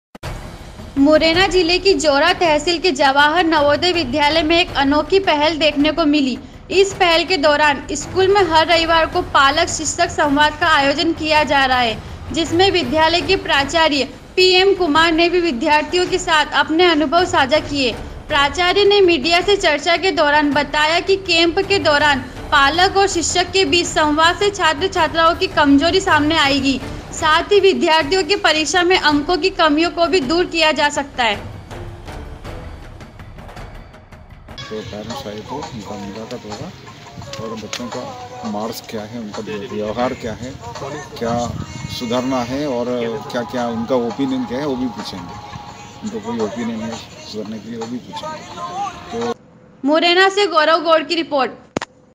मुरैना जिले के जौरा के जवाहर नवोदय विघालय में दिखी अनोखी पहल, किया शिक्षक संवाद का आयोजन
मुरैना जिले के जौरा के जवाहर नवोदय विद्यालय मालनपुर जौरा में एक अनोखी पहल देखने को मिली जहां पालक शिक्षक संवाद का आयोजन प्रत्येक रविवार को किया जा रहा है, जिसमें विद्यालय के प्राचार्य श्री कुमार पी.एम द्वारा  विद्यार्थियों की क्लास, अनुभव भी साझा किये, उन्होंने मीडिया से चर्चा करते हुए बताया की कैंप से पालक शिक्षक संवाद से छात्र एवं छात्राओं की कमजोरी को पकड़ा जा सकता है और उन्होंने कहा बच्चों के मार्क्स कम आते हैं तो उसे सुधारा जा सकता हैं।
मुरैना से गौरब गौड़ की रिपोर्ट